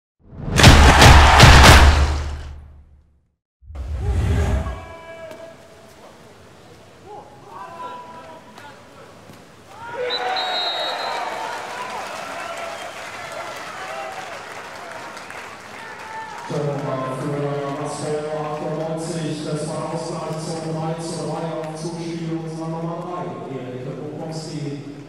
TOR DES TAGES - SUPERCUP FINALE WASPO98 vs SPANDAU 04
Das starke Kombinationsspiel von Radovic, Bukowski und Brguljan führte zum zwischenzeitlichen 3:3 im Supercup Topspiel gegen Spandau und damit auch zum ersten Titel der Saison. VivaWASPO98!